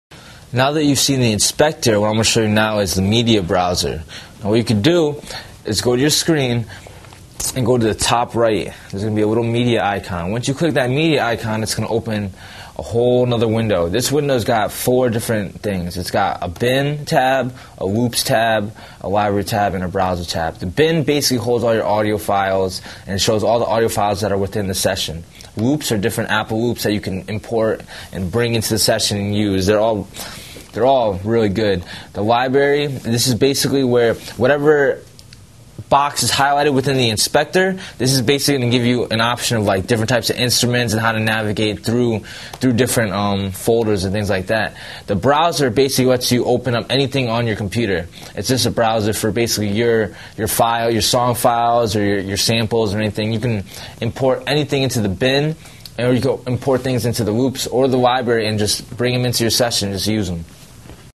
Logic Pro 8 Tutorial: Media Browser
Logic Pro 8 Tutorial: Media Browser. Part of the series: Intro to Logic Pro 8. The Logic Pro 8 media browser contains four tabs: the bin tab, the loops tab, the library tab and the browser tab. Learn about the media browser feature in Logic Pro 8 (also known as Apple Loops) software in this free video from a professional audio engineer.